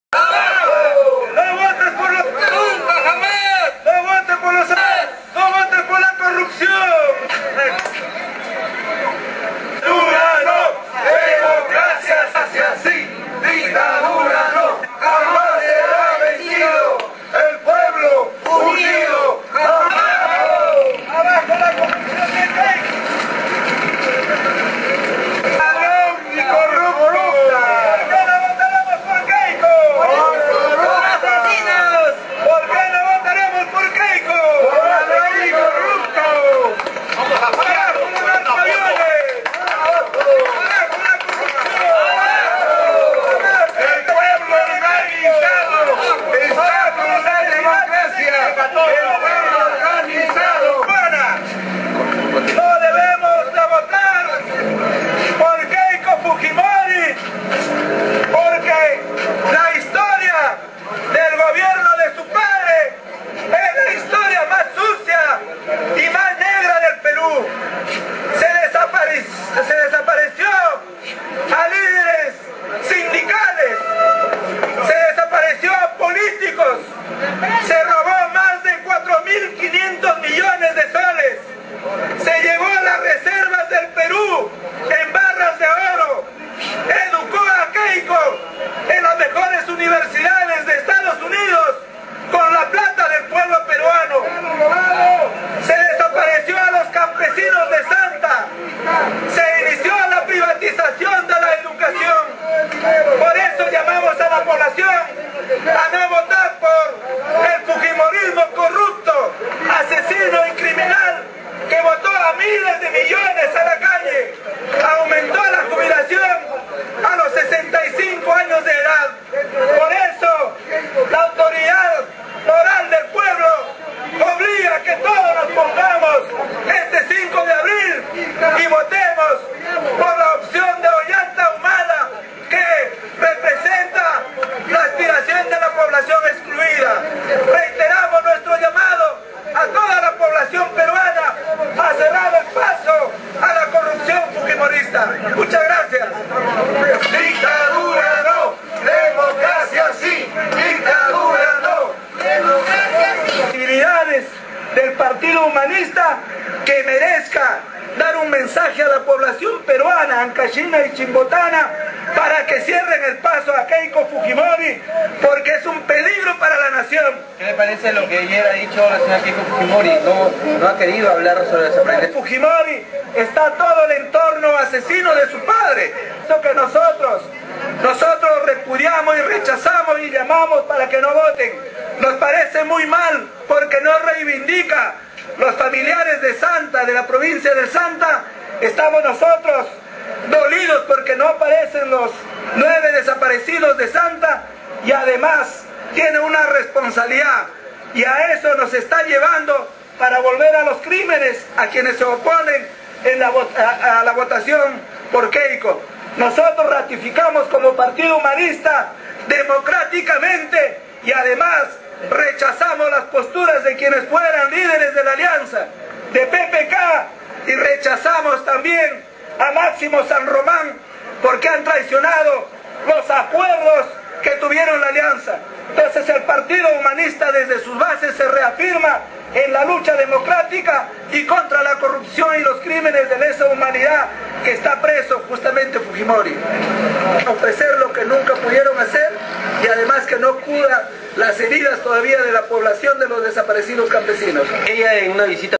KEIKO FUJIMORI - VISION POLITICA
MILITANTES DEL PARTIDO HUMANISTA  DE CHIMBOTE  DIJERON    ¡NO  A  KEIKO !